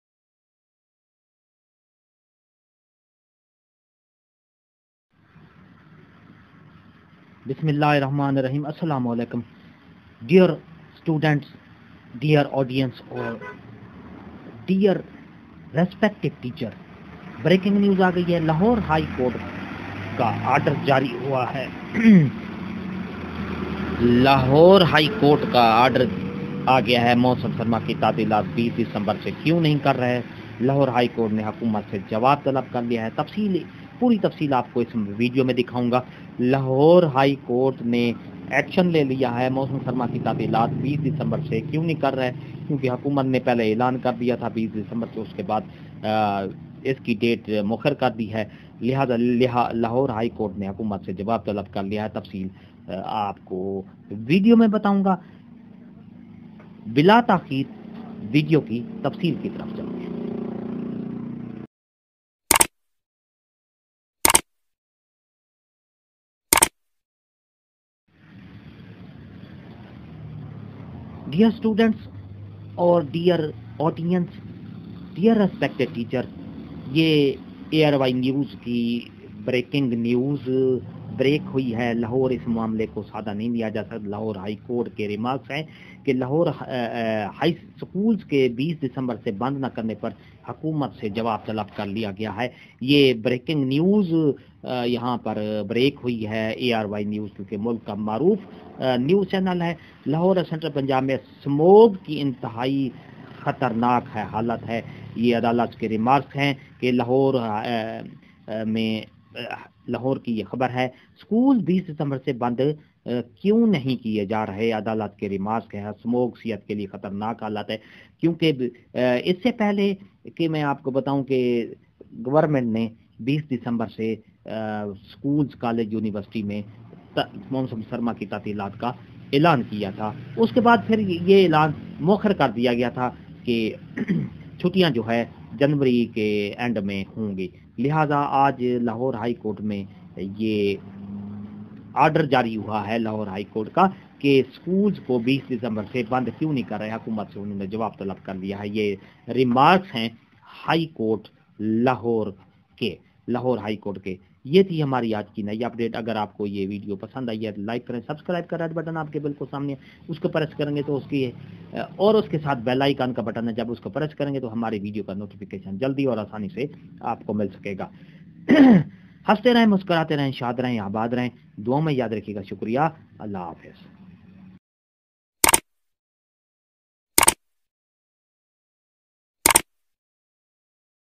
what is the order of Lahore high court,why did give the winter vaction on 20 Dec(bhatti education)
Mohammad Asghar

Jobs and service

State life